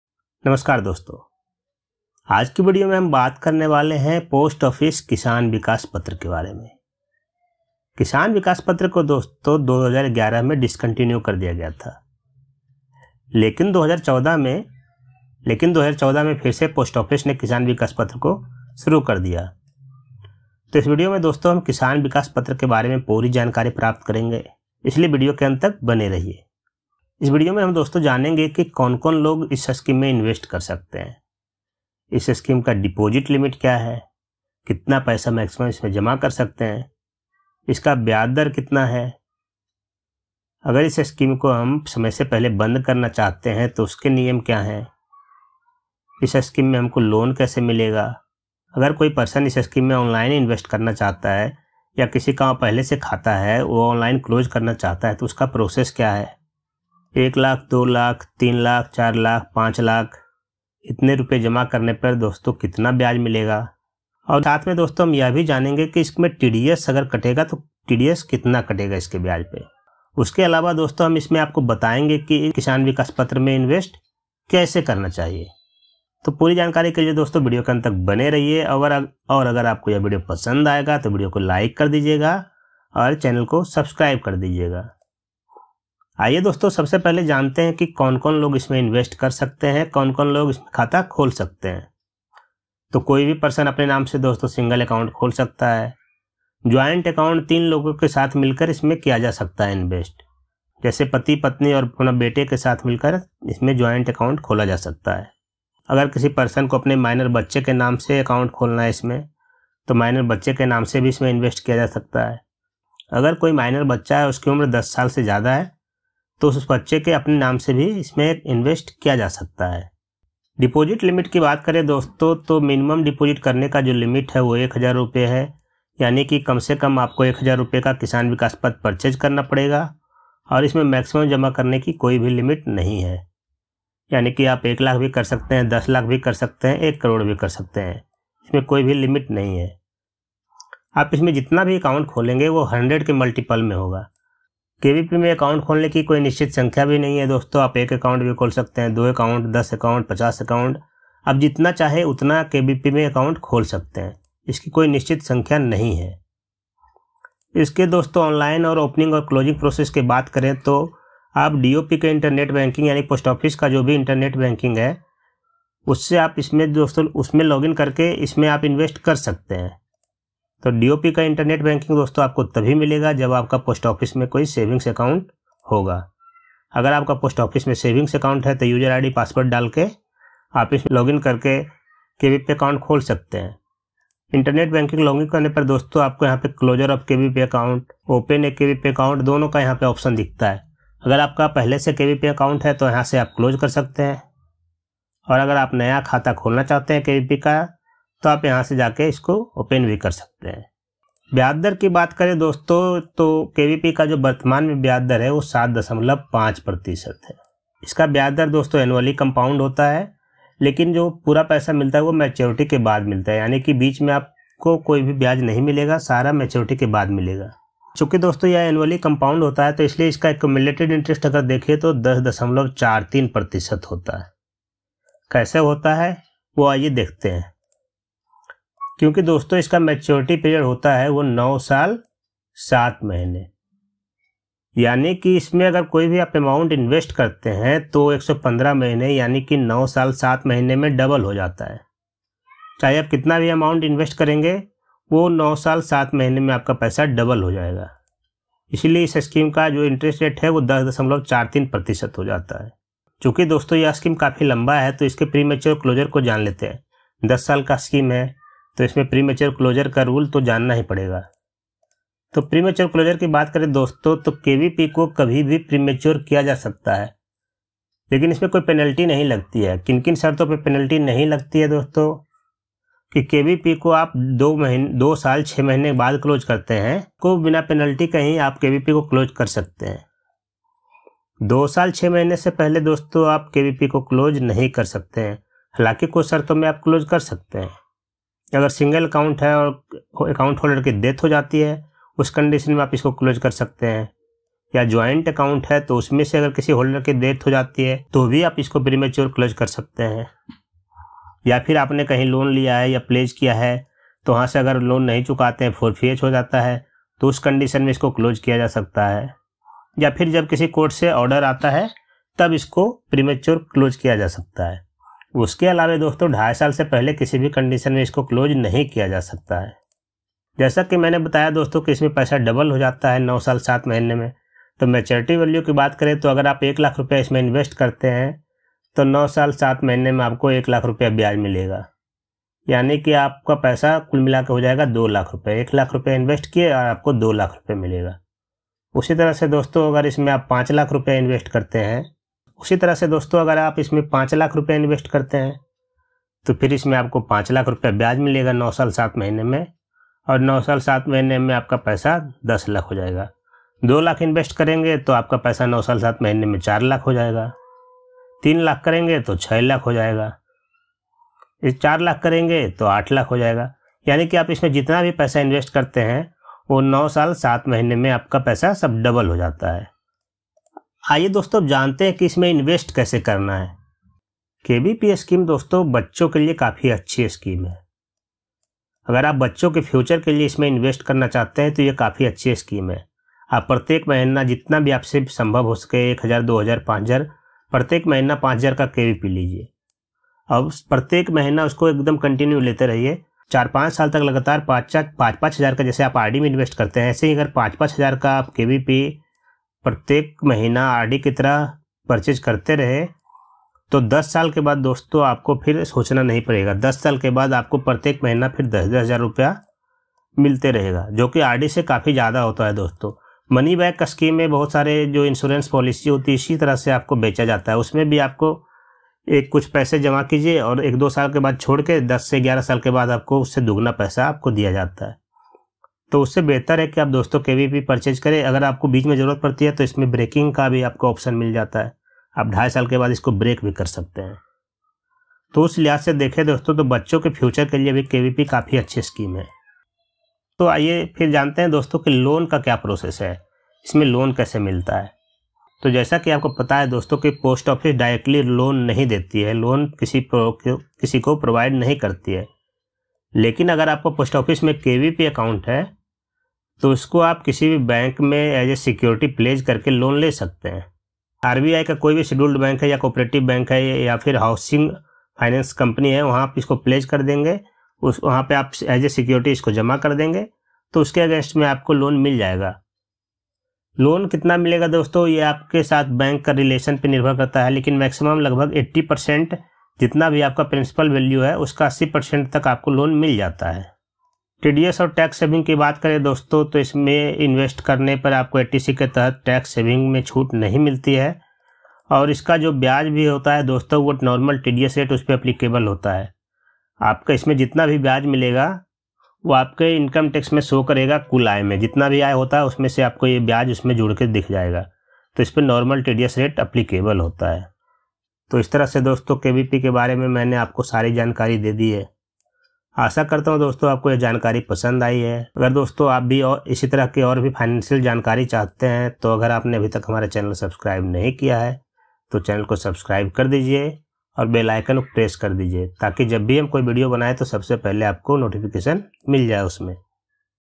Best Scheme For Child Future: Kisan Vikas Patra | किसान विकास पत्र 2023
In this video we will learn about post office kisan vikas patra. We will know that how kvp scheme is the best scheme for child future.